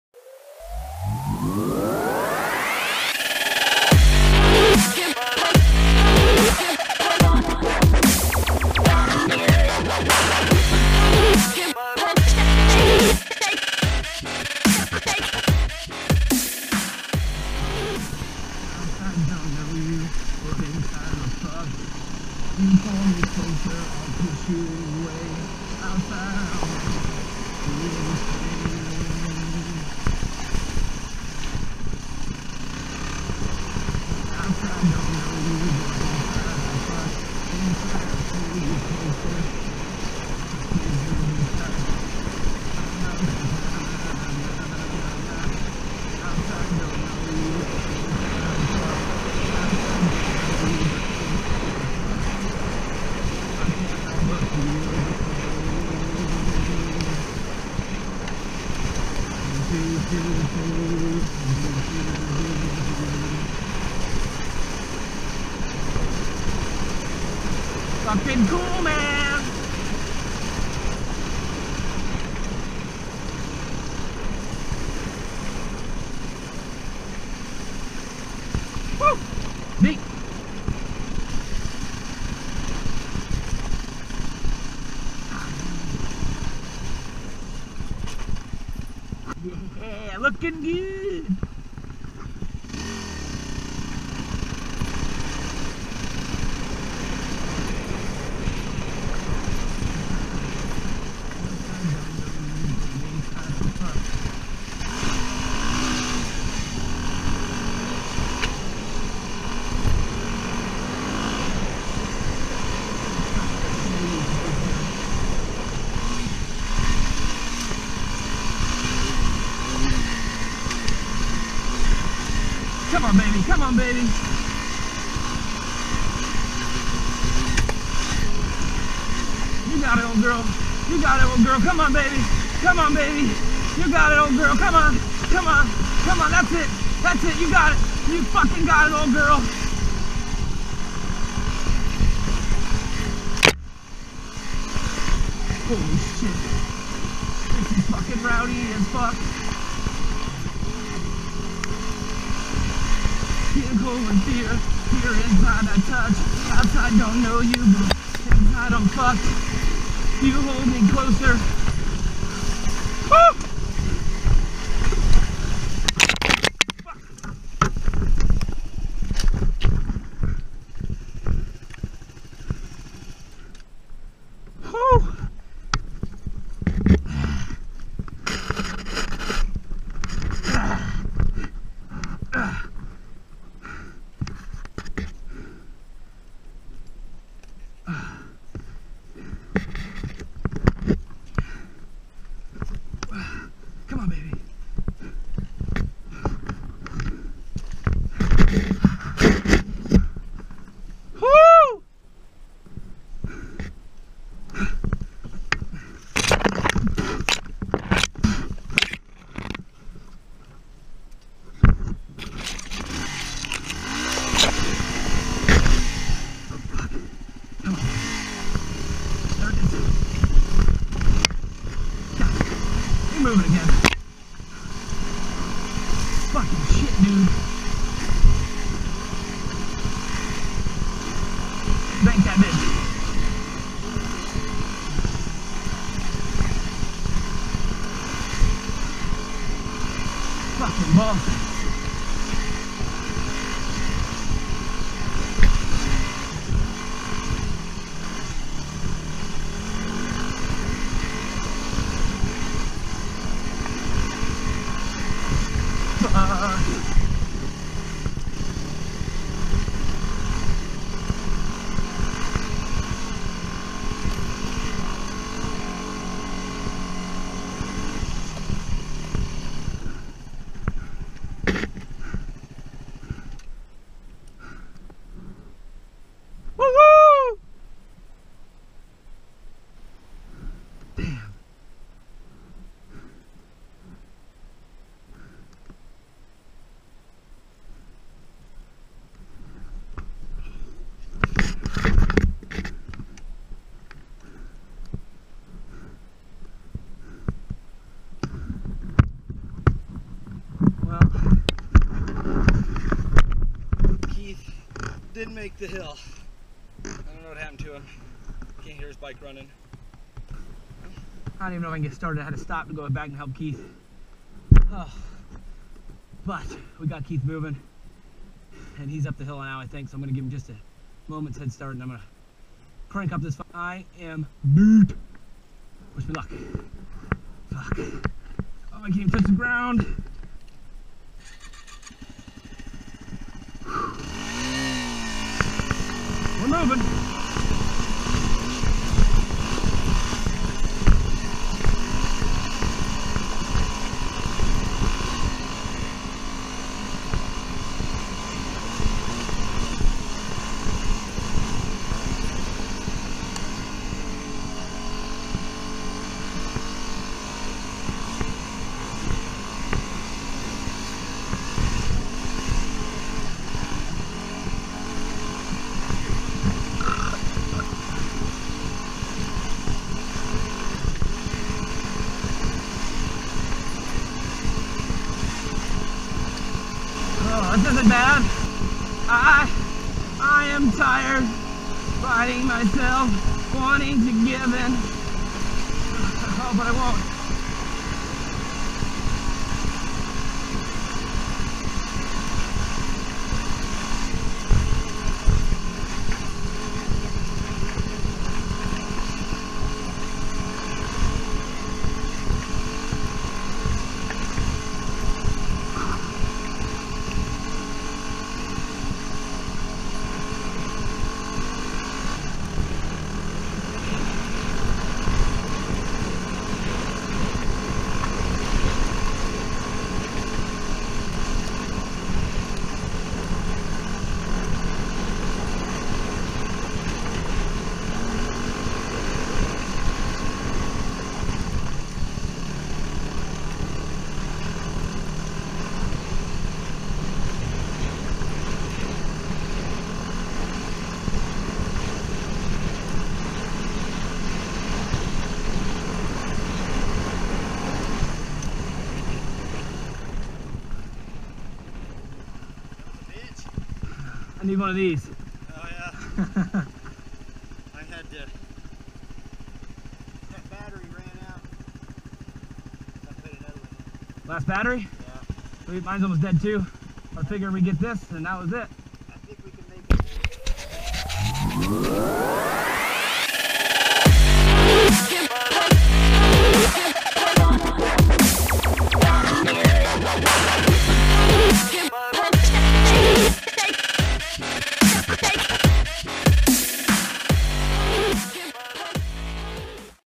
2 Wr250r's Riding on the Steese Highway in Alaska PART 7
Part 7
the monster hill climb on the road from hell
the ride out